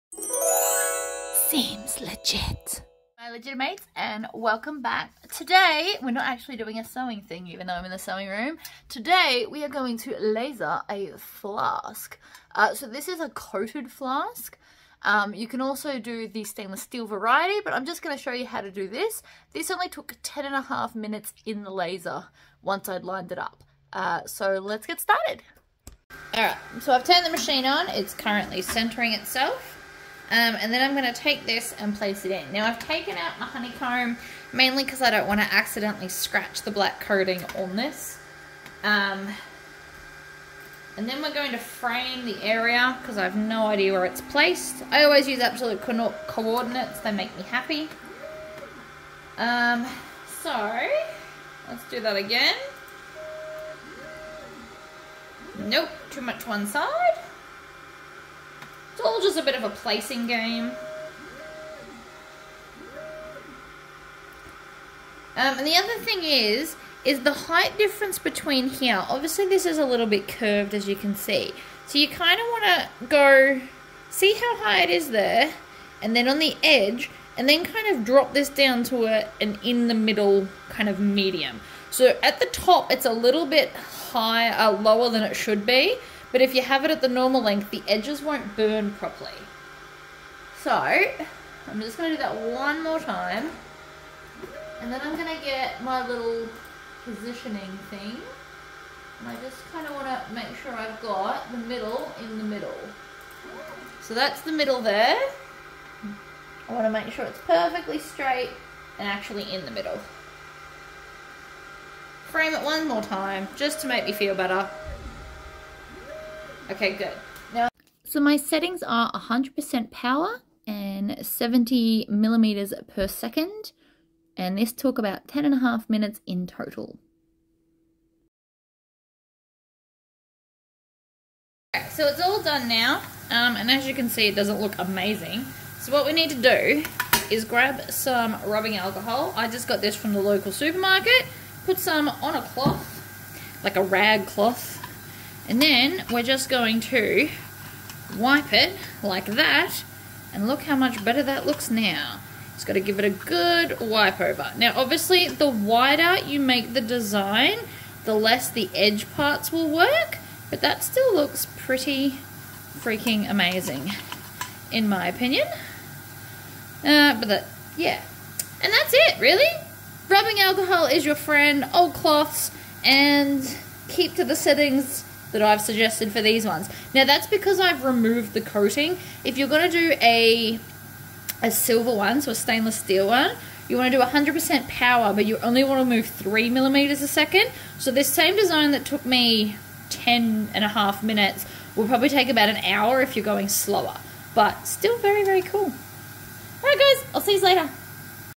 Let's laser a flask with xtool D1
I am also using the air assist, also by xtool

Flasks I bought in a pack of 10 from amazon